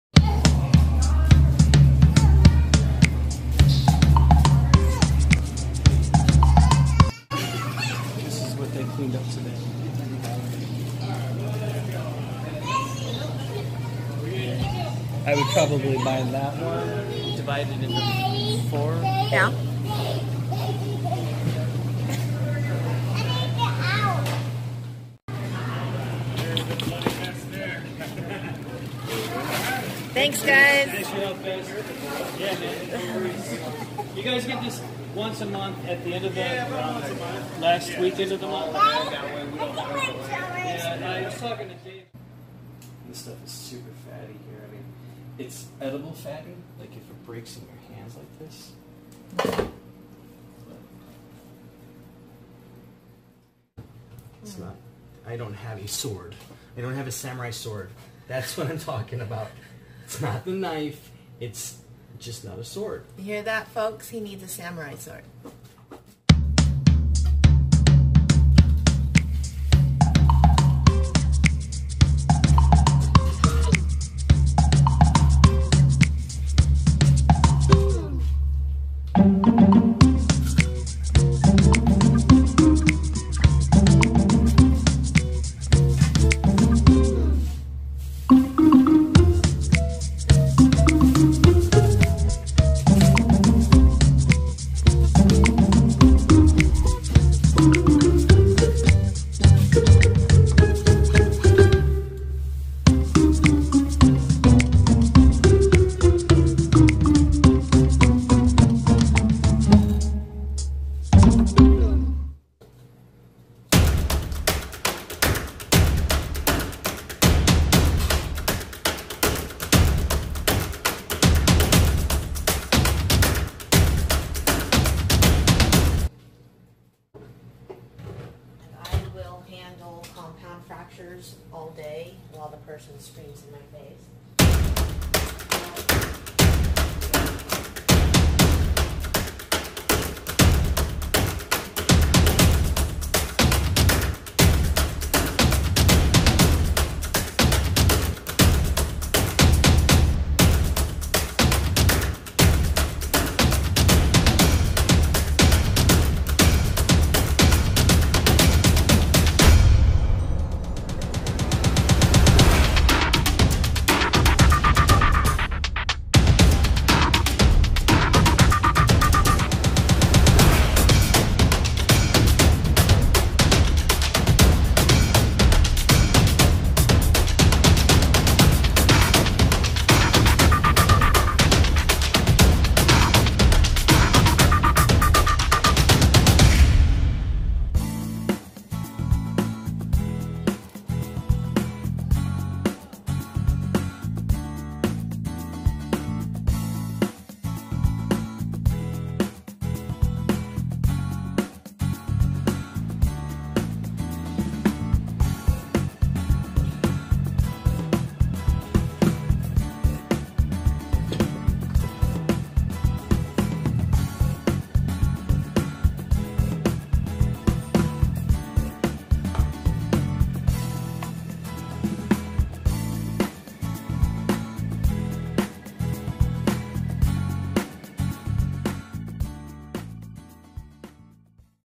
FISH SAUCE: BLUEFIN TUNA HEAD AND SPINE [PANTRY]
Chef Patrick and the family head to Uwajimaya to pick up their once monthly delivery of blue fin tuna. But, Chef doesn't want the average parts of the fish - he needs the head and spine. To acquire this he has to wait for the one time per month the store gets their blue fin tuna delivery. Chef's unique fish sauce takes much time, patience, and effort to create.